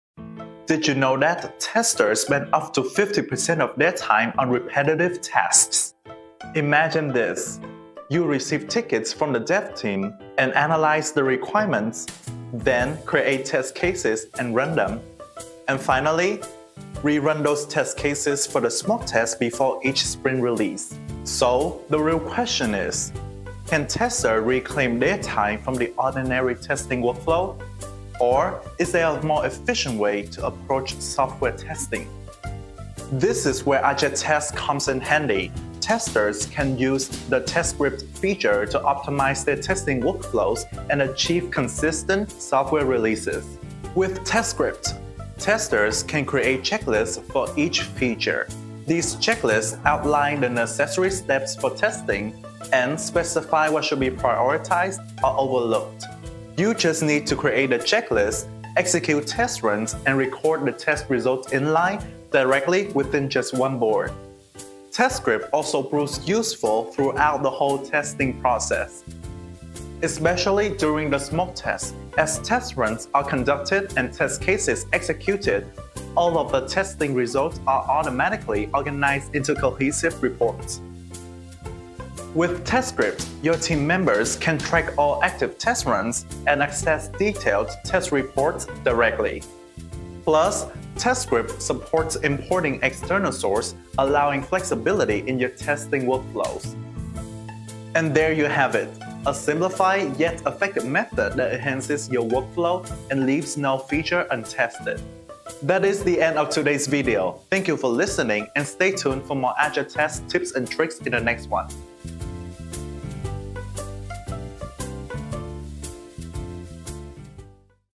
Test Scripts - Improve Testing Workflow with Jira | AgileTest Use Case
Can testers reclaim their time from the ordinary testing workflow? Or,  Is there a more efficient way to approach software testing? This is where AgileTest - Test Scripts comes in handy...

Sections:
00:00 Testers Dilemma
00:07 Testing Workflow
00:21 Help of Jira
00:43 Test Scripts
00:56 Test Runs
01:04 Smoke Test Aid
01:16 Reports
01:30 Import Test Scripts
01:37 Conclusion